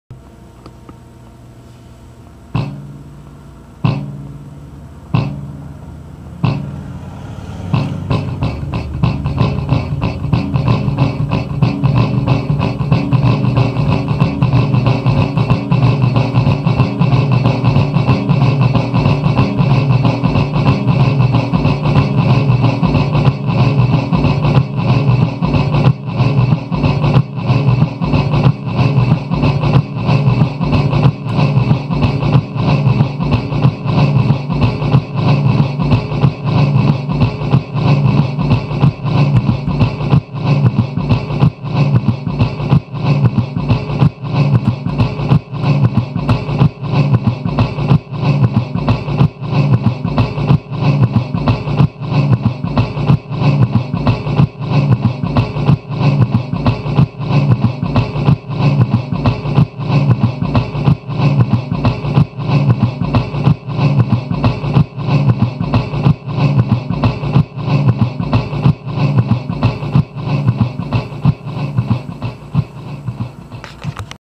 piezo + metal + wood + delay
goofing around with a couple of piezo pickups, a metal cigarette case and a wooden box that a watch came in.